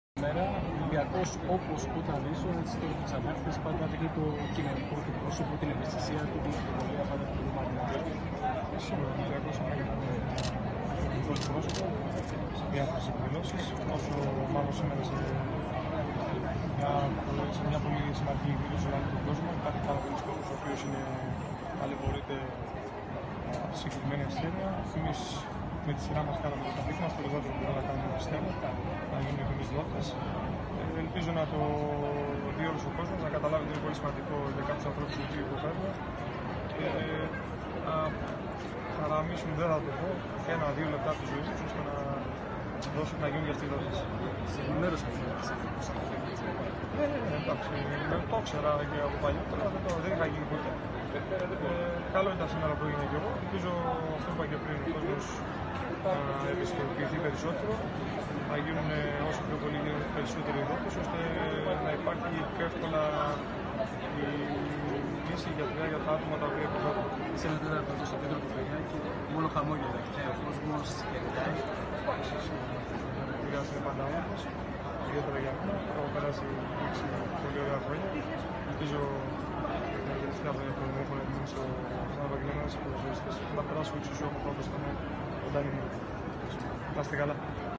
Ο Βασίλης Τοροσίδης στο κέντρο Λήψης Δειγμάτων Εθελοντών Δοτών Μυελού των Οστών της κλινικής ΡΕΑ
Ο διεθνής παίκτης του Ολυμπιακού μίλησε για την προσπάθεια που γίνεται ώστε να βρεθούν νέοι εθελοντές δότες.